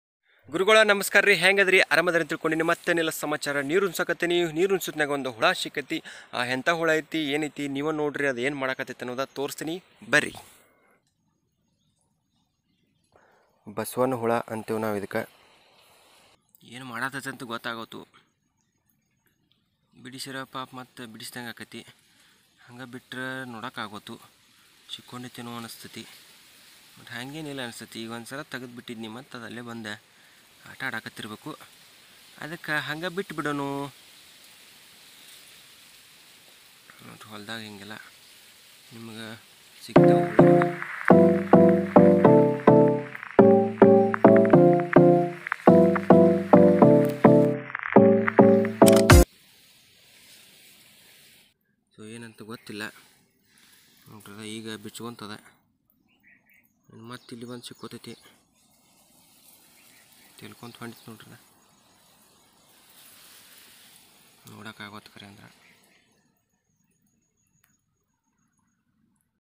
ಹೊಲದಲ್ಲಿ ನೀರ ಹಾಸುವಾಗ ಸಿಗುವಂಥ ಈ ಹುಳ ಎನ್ ಮಾಡ್ತಿದೆ ನೋಡಿ🏝️ Mr Krj Vlogs
I hope u will enojoy